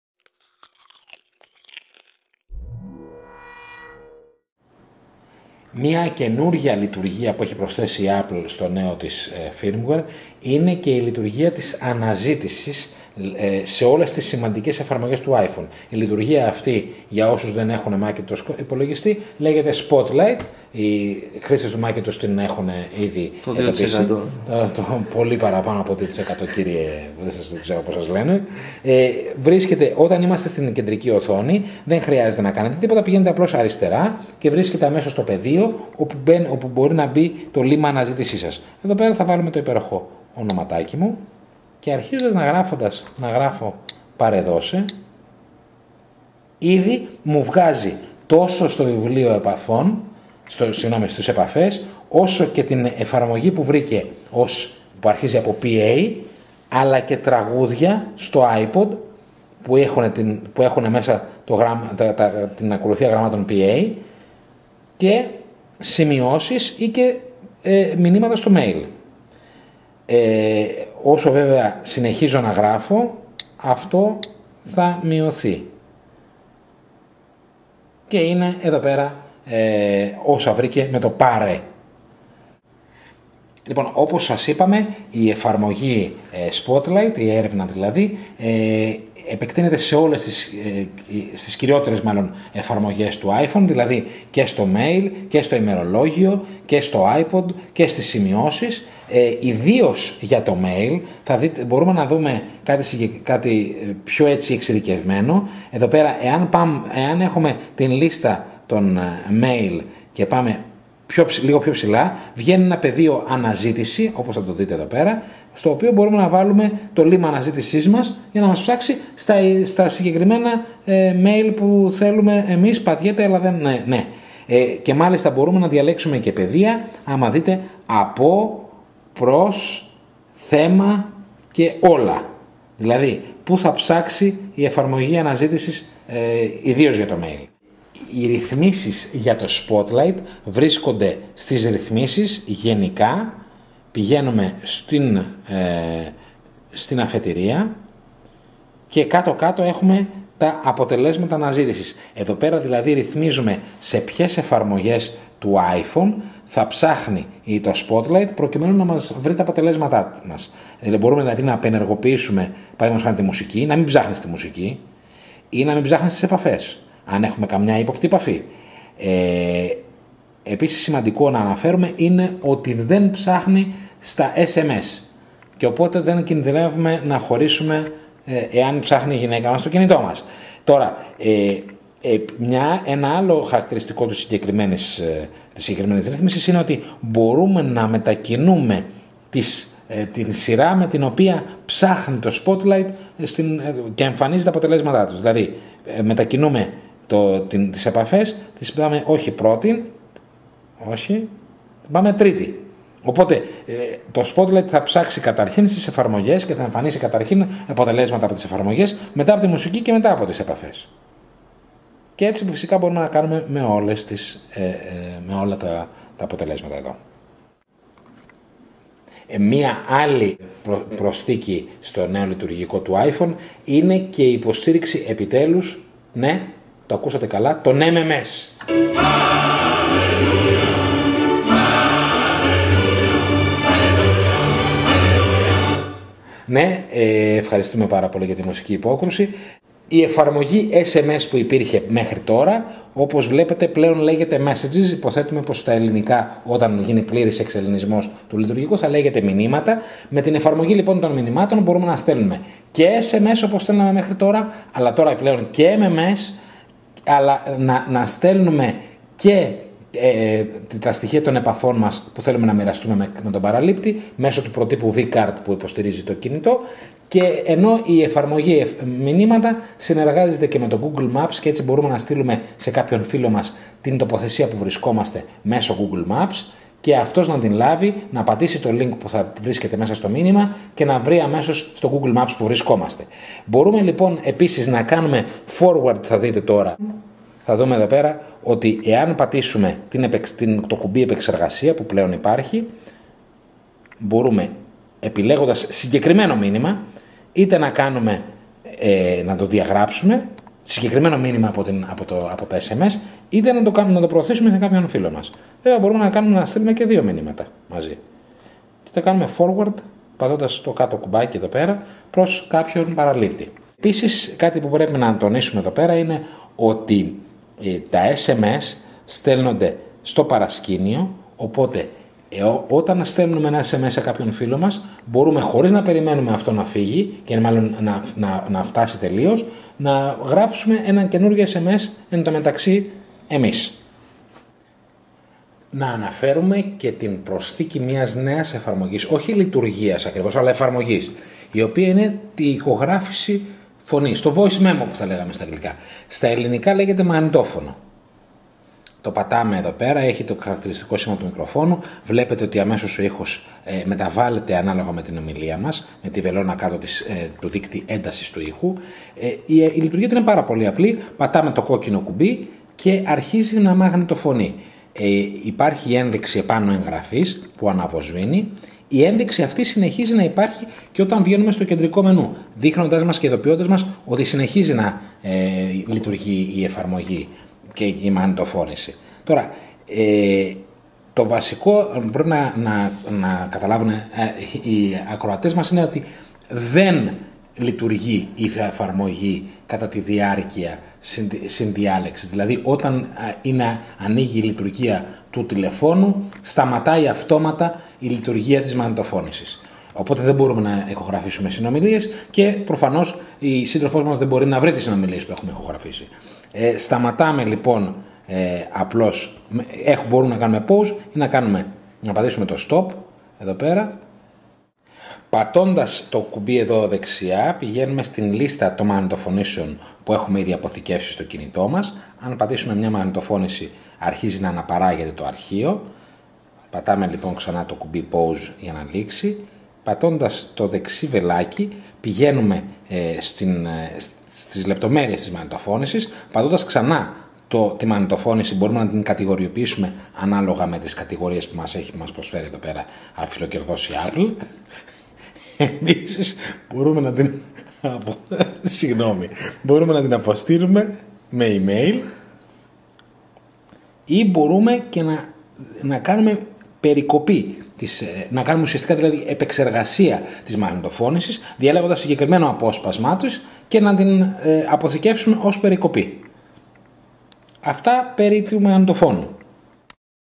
iPhone OS 3.0 beta - Spotlight/MMS/ Voice Memos (3/3)
Το τρίτο και τελευταίο μέρος της βιντεοπαρουσίασης μας για το νέο firmware του iPhone που θα κυκλοφορήσει τον Ιούνιο του 2009